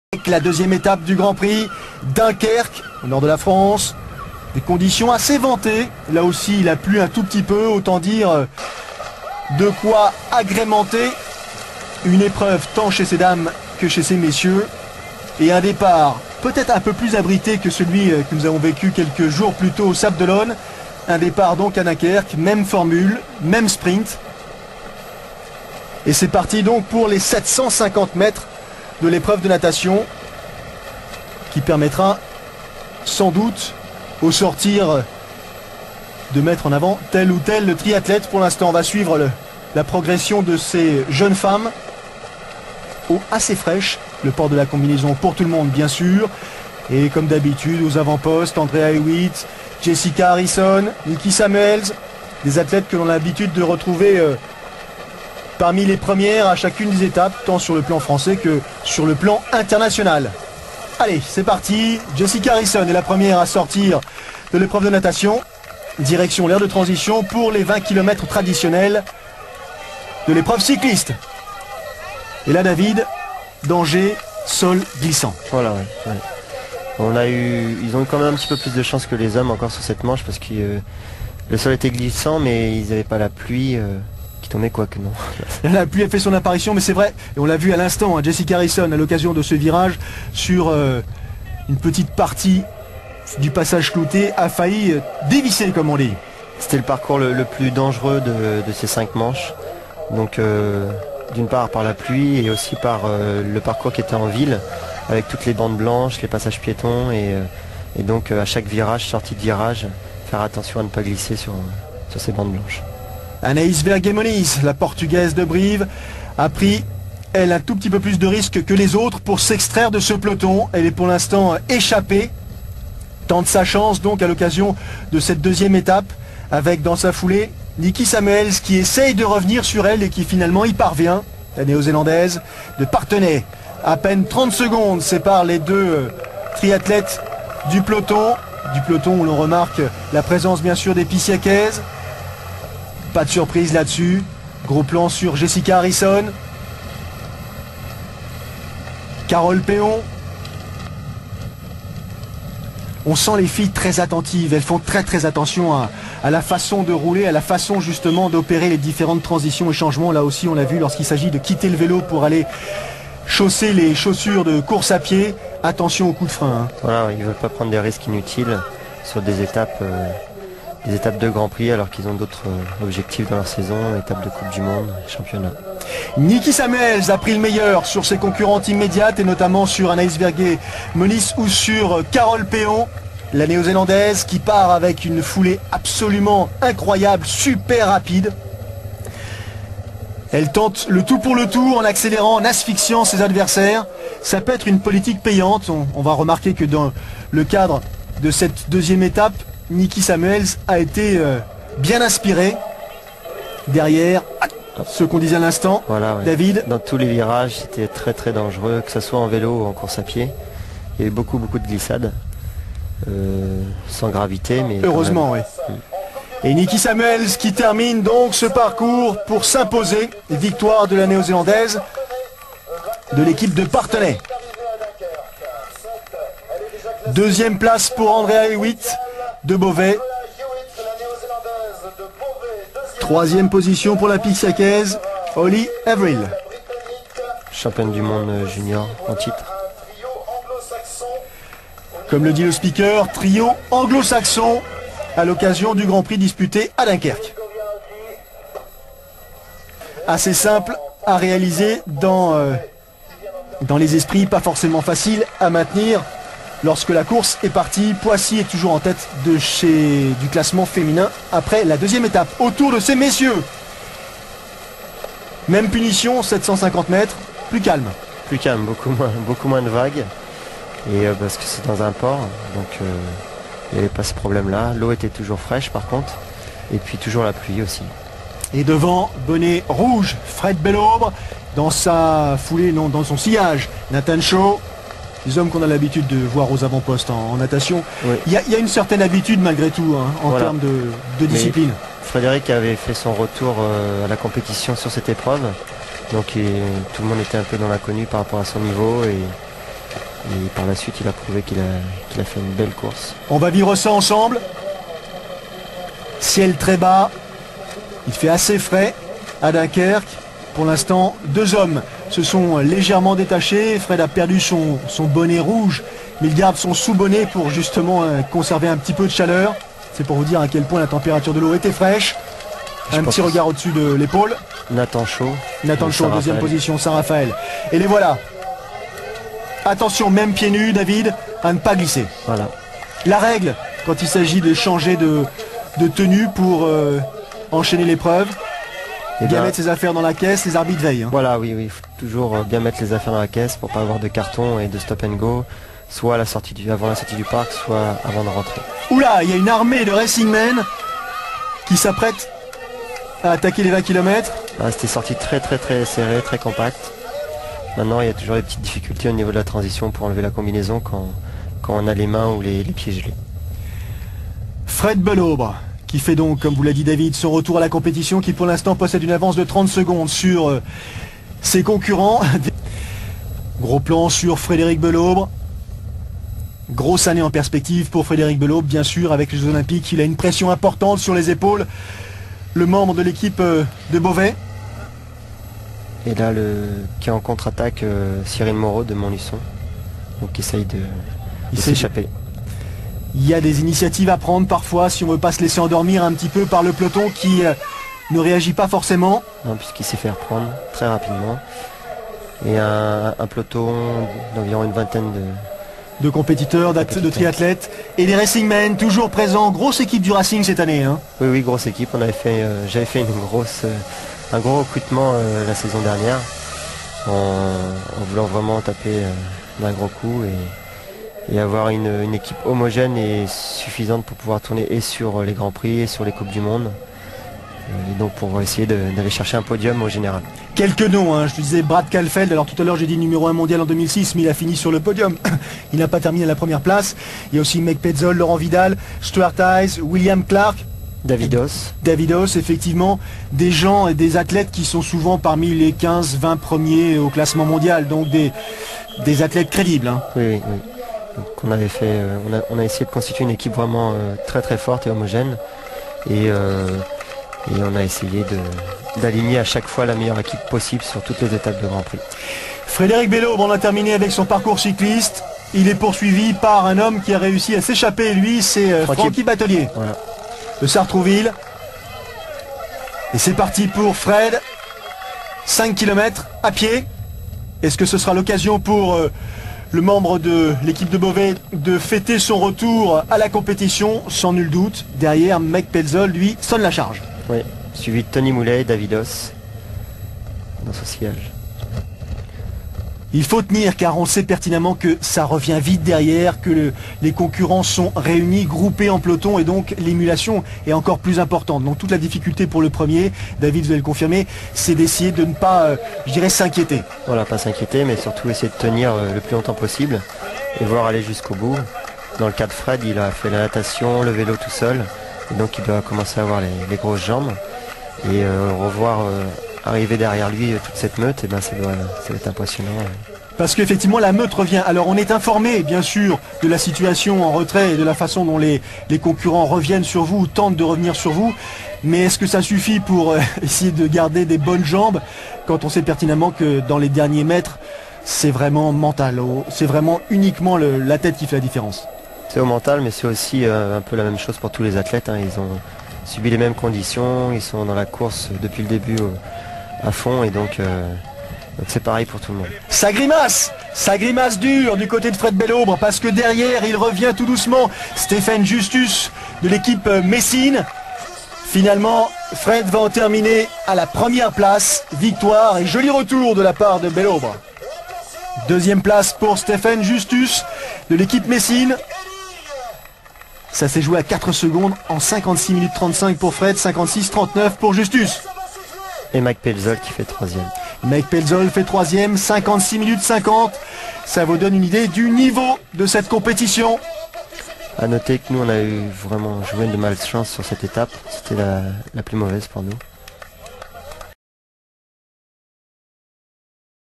triathlon gp Dunquerke 2007
Triathlon gp D1 de Dunquerke 2007.
images sport +, commentaire vincent bachelot et david bardi.